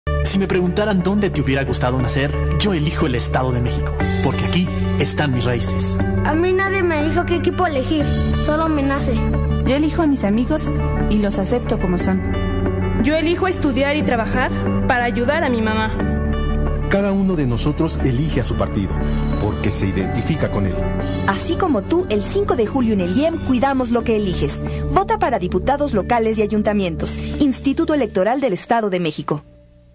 Comerciales mexicanos: IEEM 2009
Campaña cuando fueron las elecciones para diputados locales y de ayuntamiento en el Estado de México